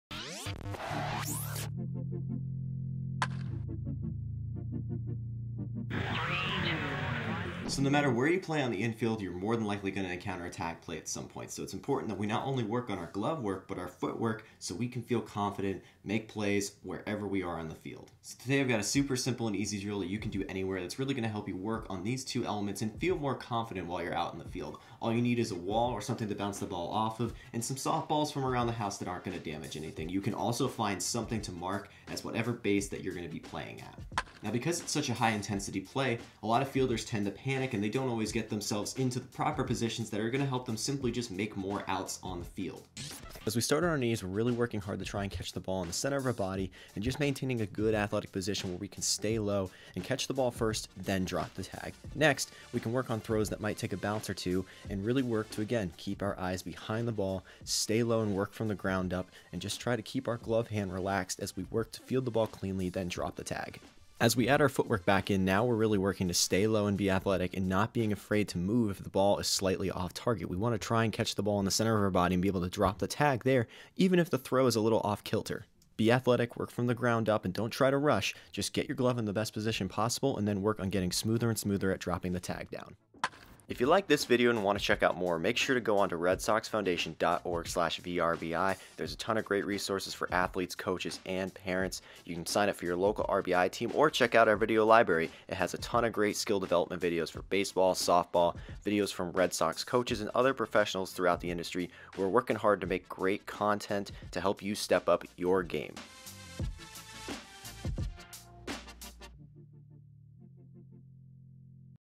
vRBI: Tag Plays
As part of vRBI presented by Franklin Sports, this at home drill will help any player work on their tags!